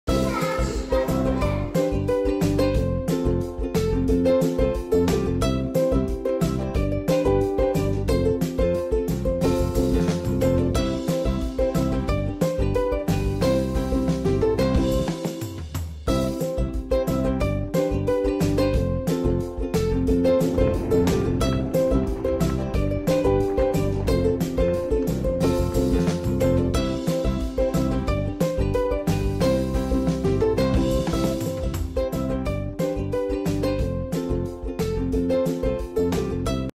Borboleta 🦋 com rolinho de papel higiênico
Borboleta com  papel higiênico
Recurso, fita creme, rolinho vazio de papel higiênico, lápis de cor